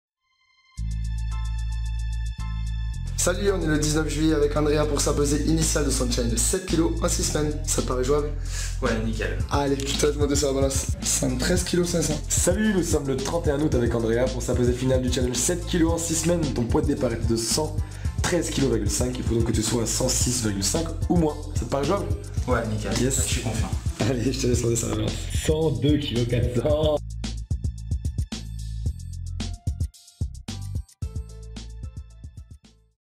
Transformation physique rapide Bordeaux
Andrea a perdu 11,100 kgs en 6 semaines à la Training Academy